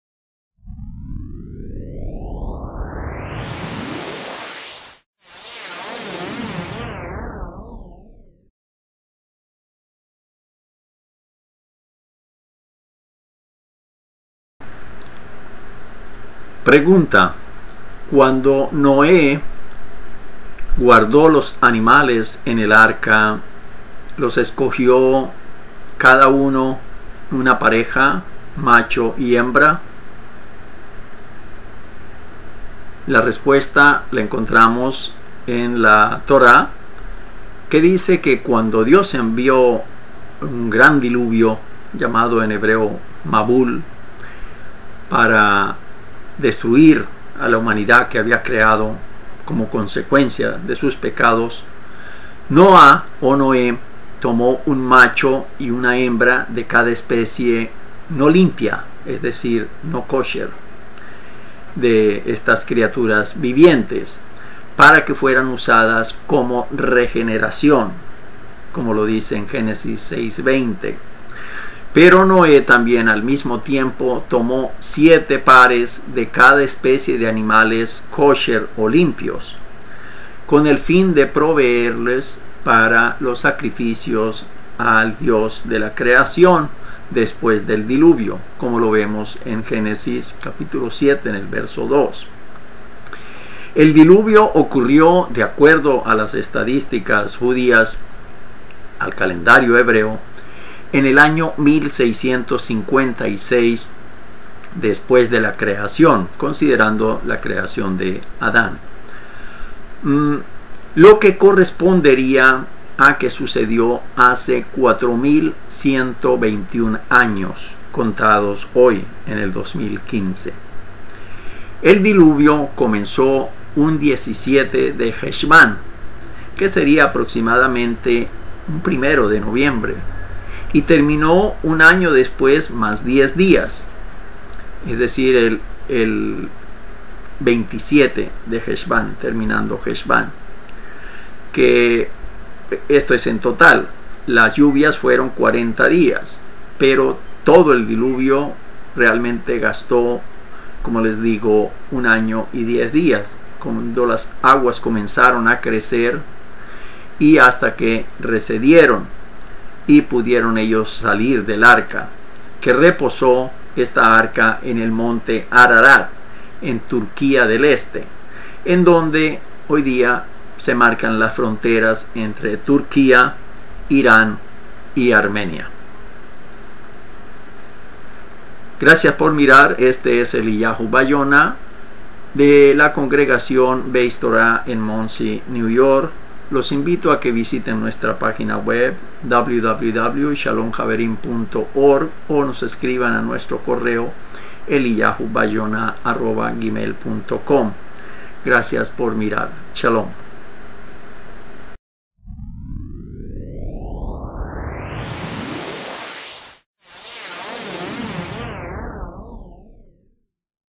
EL DILUVIO UNIVERSAL- Cuántos animales metió Noé en el Arca?
Metió Noé en el Arca solamente parejas de animales? Cuando ocurrió el Diluvio?
El Maestro Eliyahu BaYona es Periodista Profesional y Director de la Organización ShalomHaverim y miembro de la Sinagoga Bais Toráh en Monsey, New York